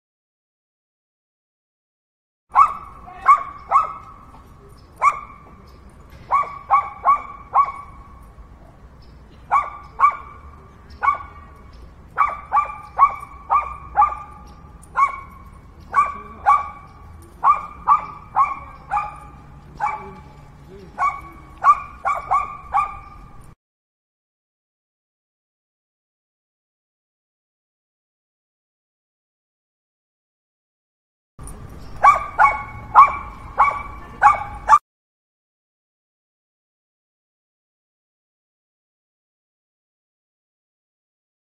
When Animals Attack a Unicyclist ...
Just a funny moment from Alamo Square Park.
Sound effects added for comic relief:
Iwan 'qubodup' Gabovitch - Tiny Dog Barking in Park